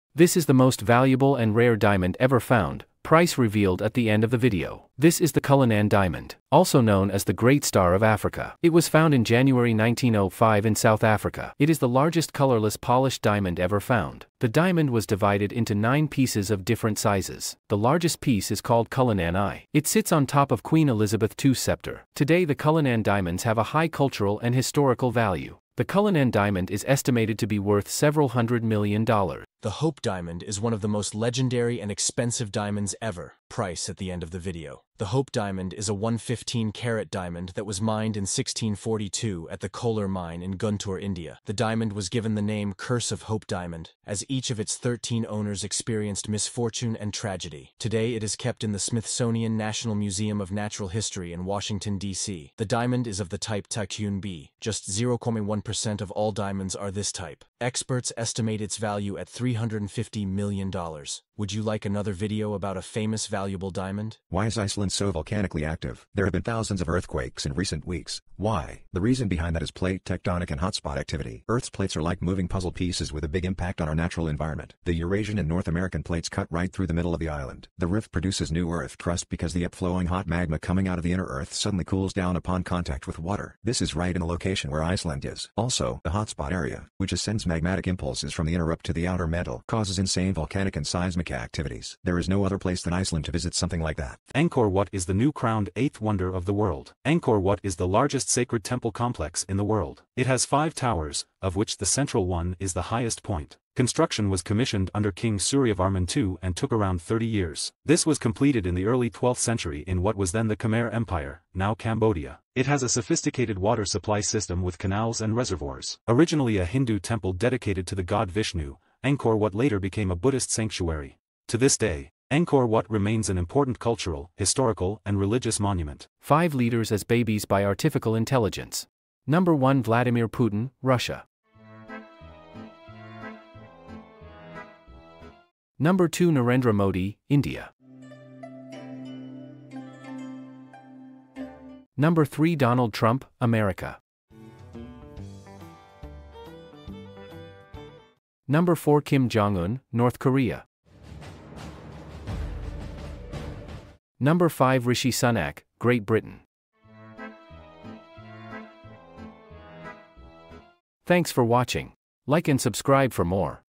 Short Compilation #1 I Bailey I English I history I geology I
This is my first compilation of my five selfmade shorts. Enjoy!

Number 1 The famous Cullinan Diamond and his story
Number 2 The interesting and cursed story/legend about the Hope Diamond
Number 3 The geological processes on Iceland and its current effects
Number 4 The story about the new called "8th World Wonder" Angkar Wat
Number 5 Five leaders AI-reimaged into babies with fitting themes

If you like those compilations give me your feedback if I should continue :)